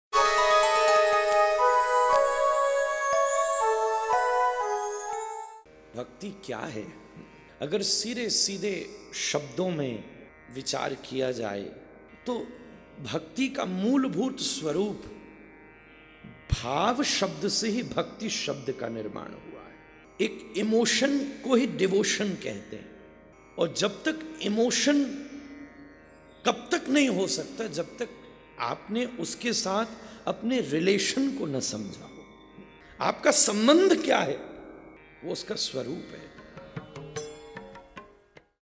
आचार्य श्री पुण्डरीक गोस्वामी - भक्ति का मूल क्या है - Bhakti Ka Mul Kya Hai - Pundrik Ji - सदुपदेश
आचार्य श्री पुण्डरीक गोस्वामी - भक्ति का मूल क्या है - Bhakti Ka Mul Kya Hai - Pundrik Ji - सदुपदेश - Bhaktisagartv